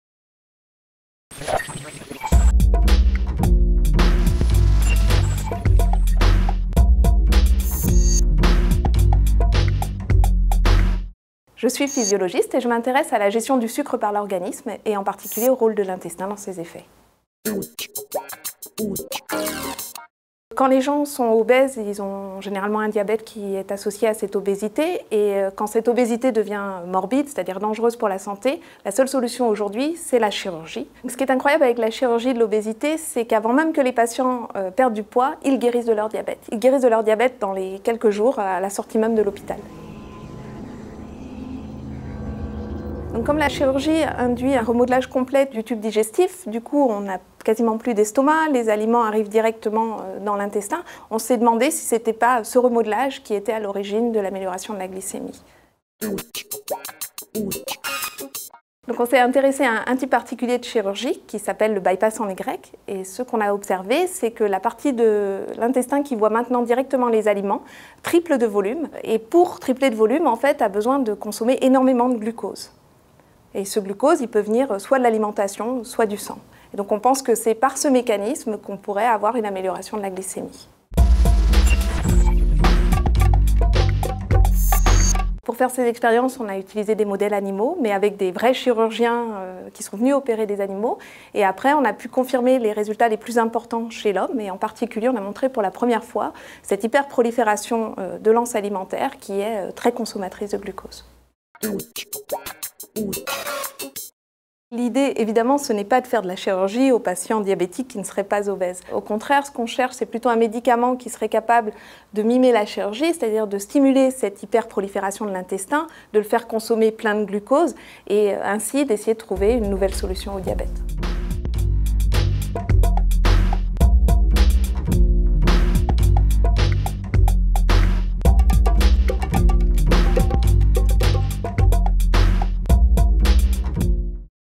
L'intestin au secours du diabète
Obésité : la chirurgie corrige le diabète avant même la perte de poids
Chaque année, l’obésité tue 2,8 millions de personnes dans le monde. Face à un poids morbide - et au diabète de type 2  qui en est une conséquence directe - la chirurgie s’impose en dernier recours. Étrangement, chez les patients opérés, le diabète disparait avant et indépendamment de la perte du poids. Des chercheurs de l’Inserm, avec l’aide des médecins de l’hôpital Bichat, AP-HP, révèlent que l’intestin remodelé par l’intervention pourrait être à l’origine de ce meilleur contrôle de la glycémie. Cette étude est parue dans Gastroenterology.